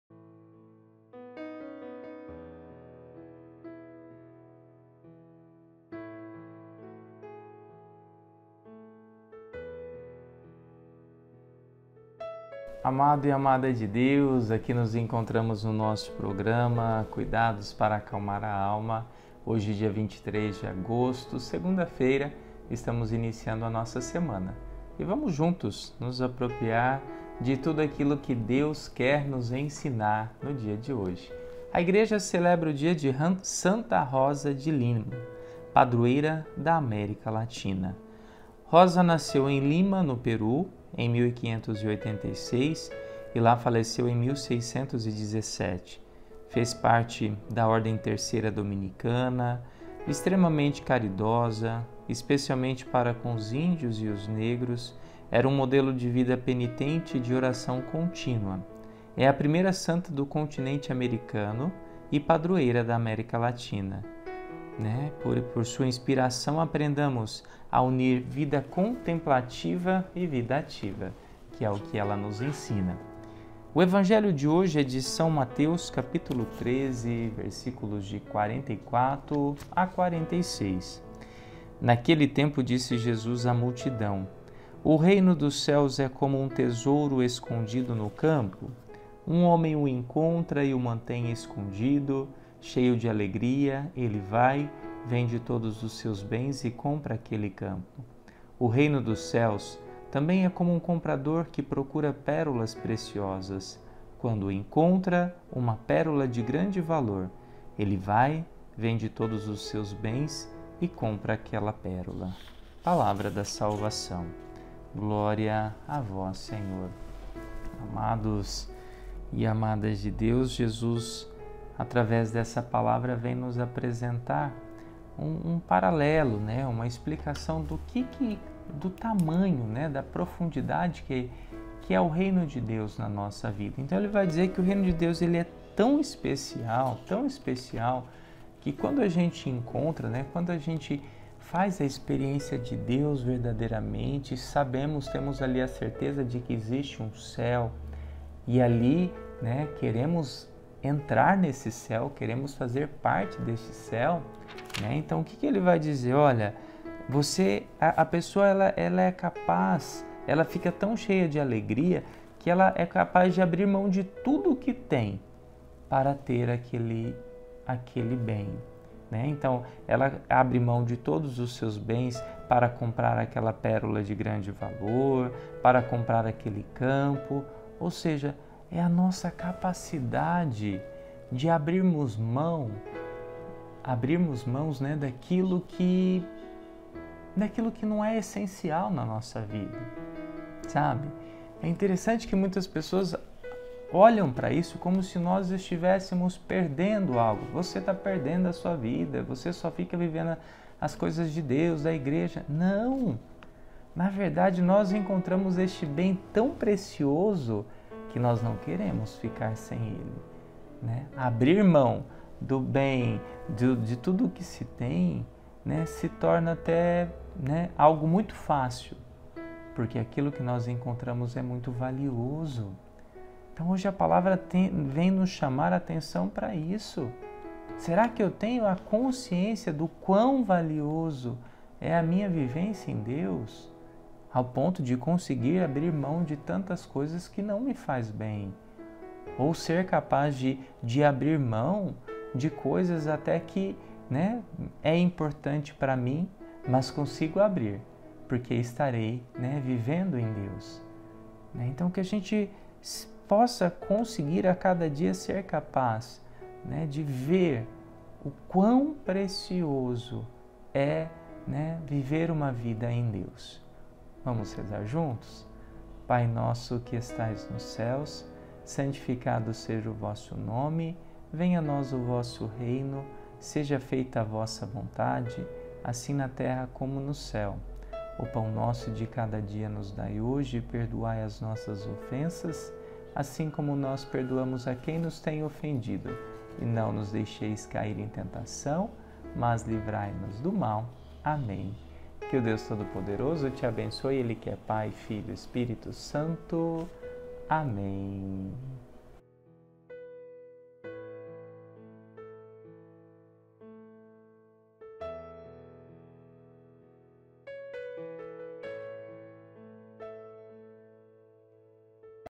Homilia Diária - Segunda-Feira - 21ª Semana do Tempo Comum
Nas duas mensagens deste Evangelho, Jesus nos apresenta o reino dos céus como algo muito valioso que alguém busca, e encontra. Na nossa caminhada aqui na terra, desde cedo saímos à procura da felicidade nos mais diversos lugares, nas ocasiões e situações que nos atraem, assim como também no amor das pessoas, na realização profissional, no sucesso nos empreendimentos, etc. e nem sempre isso nos torna pessoas alegres e felizes. Ele é como um tesouro que está escondido dentro do nosso coração no meio do campo da nossa humanidade ou uma pérola preciosa que é buscada por nós, mas que só é encontrada quando nos encontrarmos conosco.